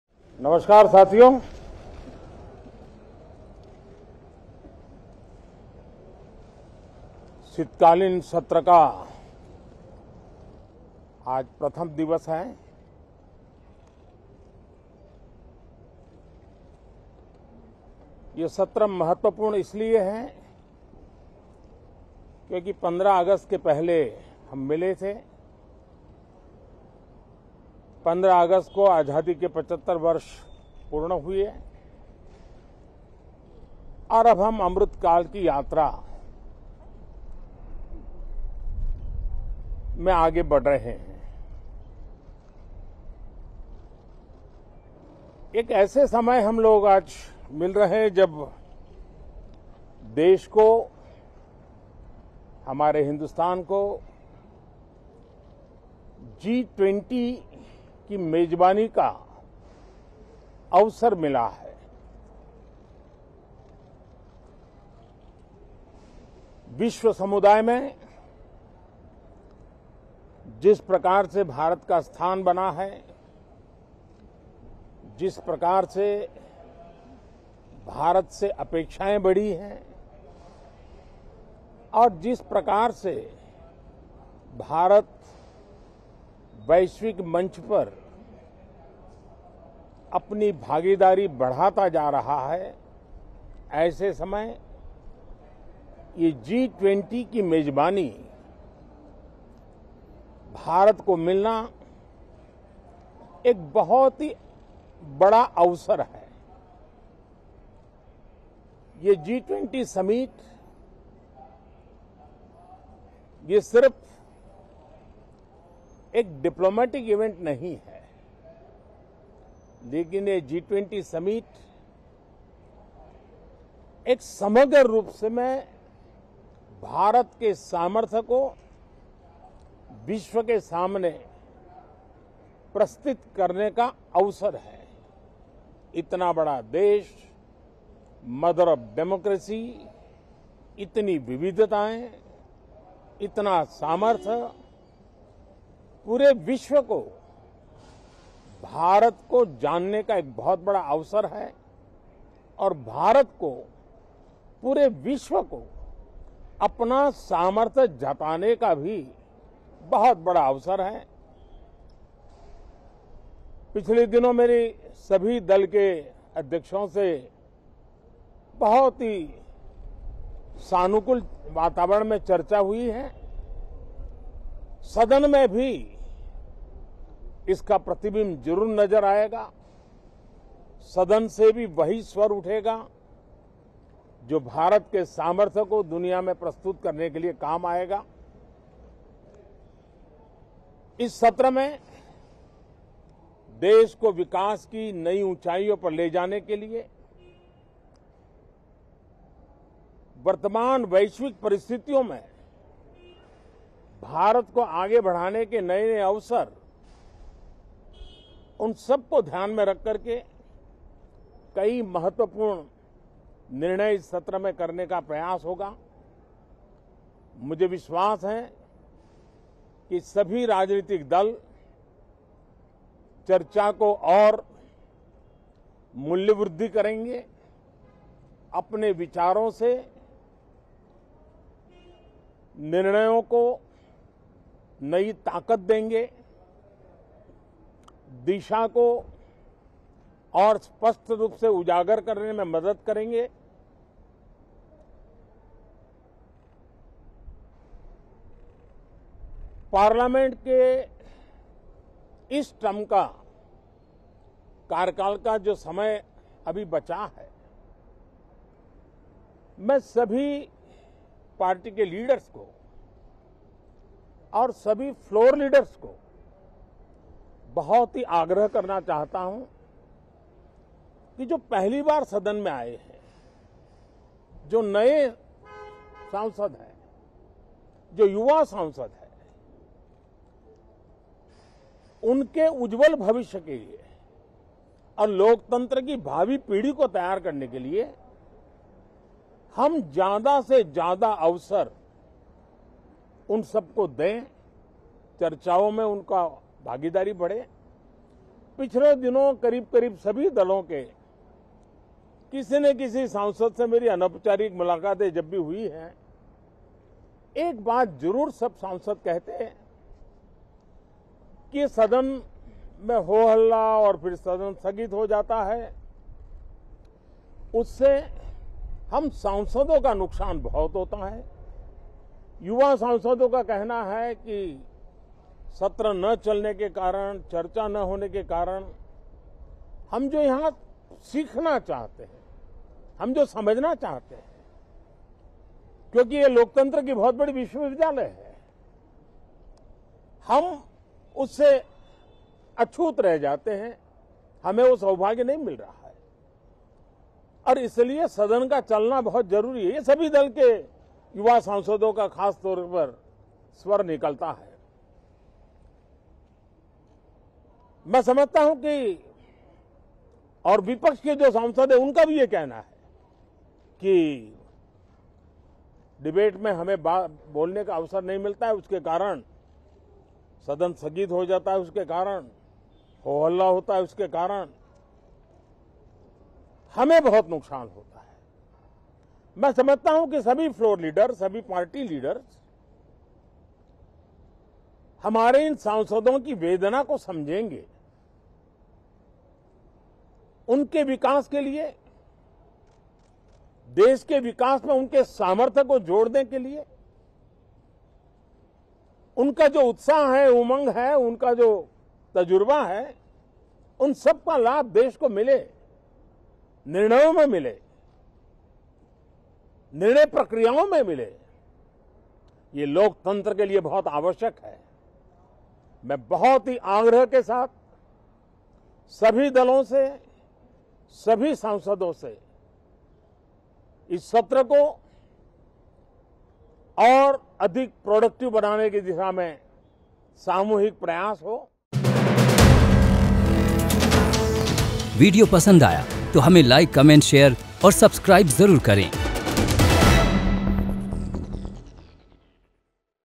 शीतकालीन सत्र की शुरुआत में सांसदों से क्या बोले PM?
संसद के शीतकालीन सत्र के पहले दिन PM Narendra Modi  मीडिया से रूबरू हुए। प्रधानमंत्री ने कहा ये सत्र कई अहम फैसलों के लिए जाना जाएगा। आपको बता दें कि संसद का शीतकालीन सत्र आज से शुरू होकर 29 दिसंबर तक चलेगा। इससे पहले 6 दिसंबर को संसद परिसर में रक्षा मंत्री राजनाथ सिंह की अध्यक्षता में सर्वदलीय बैठक हुई थी, जिसमें तमाम राजनेताओं ने भाग लिया था।